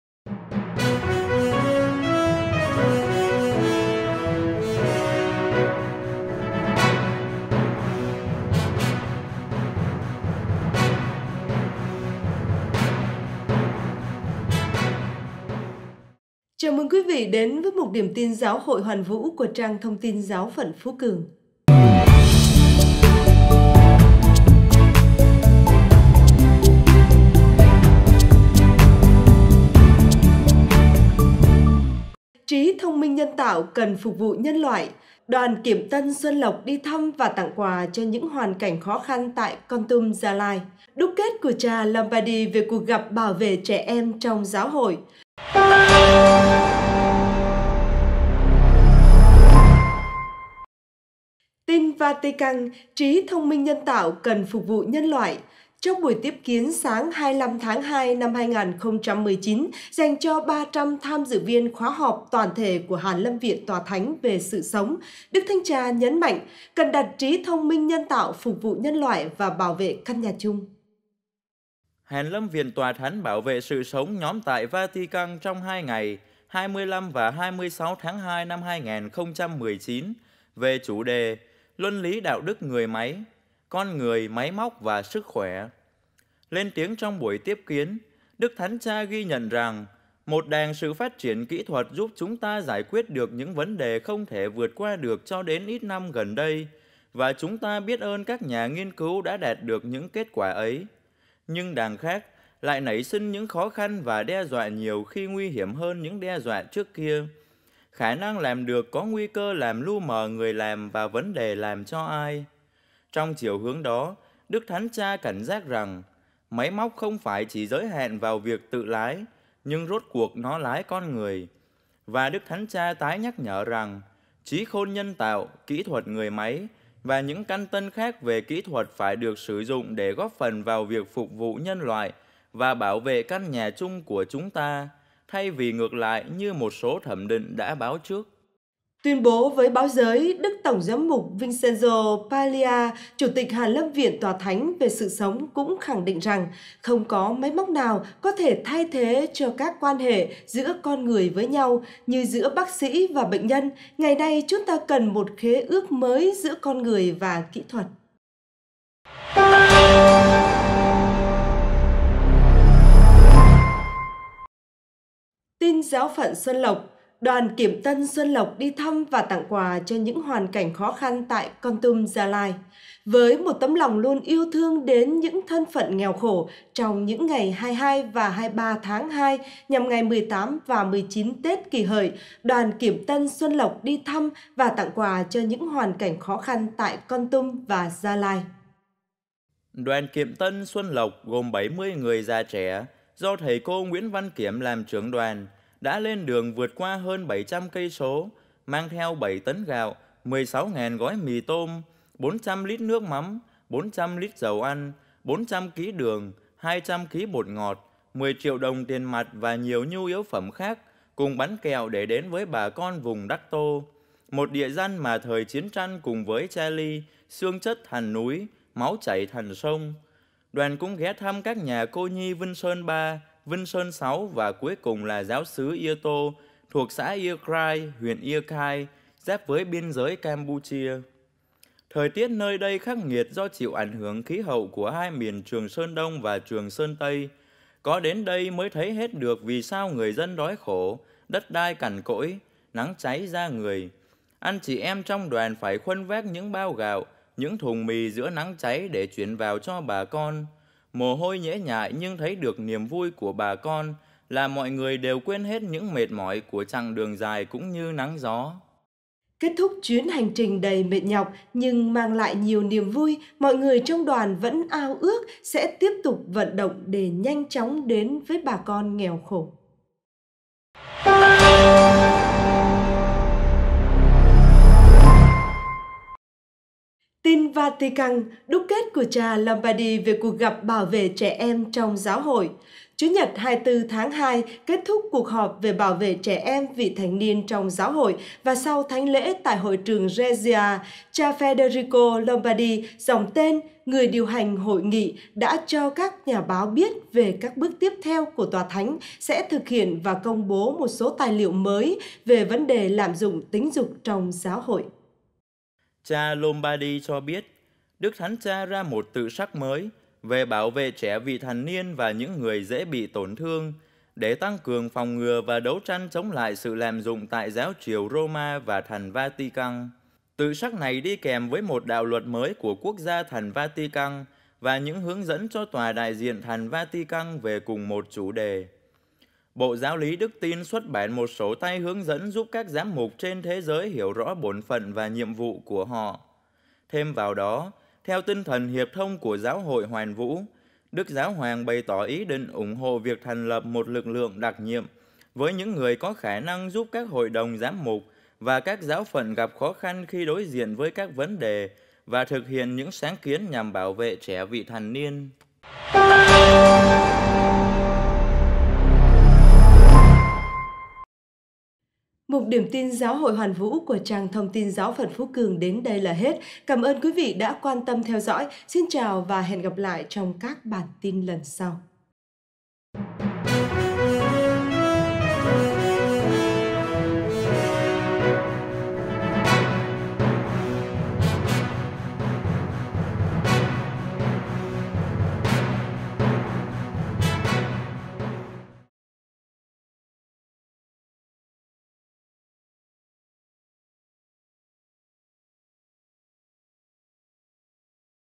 ĐIỂM TIN GIÁO HỘI HOÀN VŨ 28.02.2019
1. TIN VATICAN: Trí thông minh nhân tạo cần phục vụ nhân loại.
2. TIN GIÁO PHẬN XUÂN LỘC: Đoàn Kiệm Tân - Xuân Lộc đi thăm và tặng quà cho những hoàn cảnh khó khăn tại Kon Tum và Gia Lai.
3. TIN VATICAN: Đúc kết của cha Lombardi về cuộc gặp bảo vệ trẻ em trong Giáo Hội.